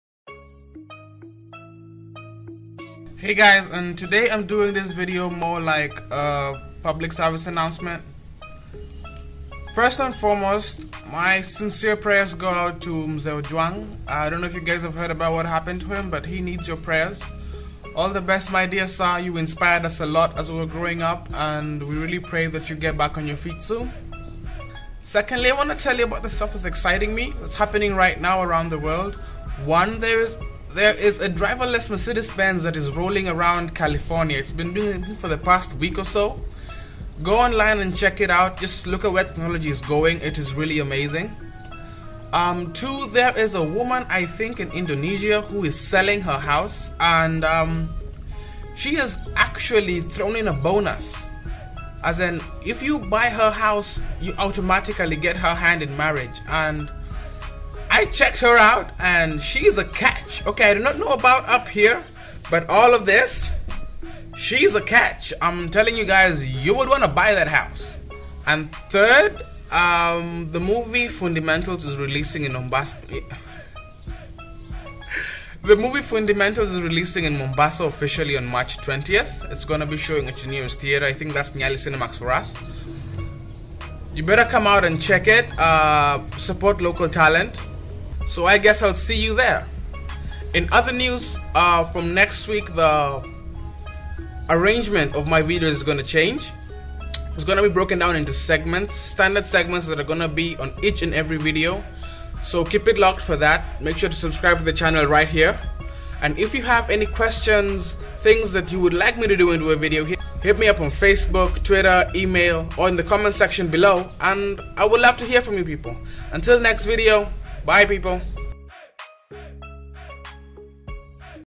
My Thoughts
Today I decided, to let you guys in abit.
There are alot of questions about who I am, I am hoping this video will give you a sense of my character and a little understanding of who I am by getting to know what interests me.

If you got something to say, thats what the comment section is for!